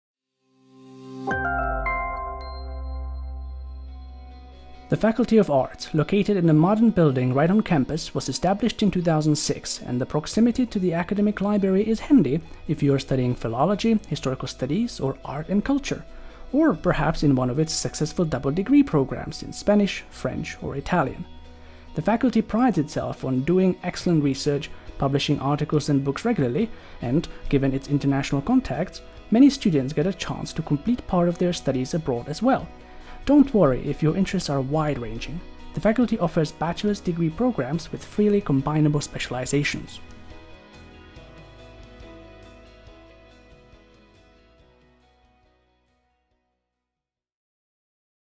Faculty of Arts of the University of South Bohemia in České Budějovice
The Faculty of Arts at the University of South Bohemia offers education in the fields of History and Philology and in the fields of Aesthetics and Art History, both in Bachelor's, Master's and Doctoral degrees. A high level of education is guaranteed by high-quality research background and extensive publishing activities of the teachers.
The Faculty consists of the Institute of Archaeology, Institute of History, Institute of Archival Studies and Auxiliary Sciences of History, Institute of Czech Studies, Institute of Art and Culture Studies, Institute of Romance Studies, Institute of Czech-German Area Studies and German Studies and Institute of English Studies.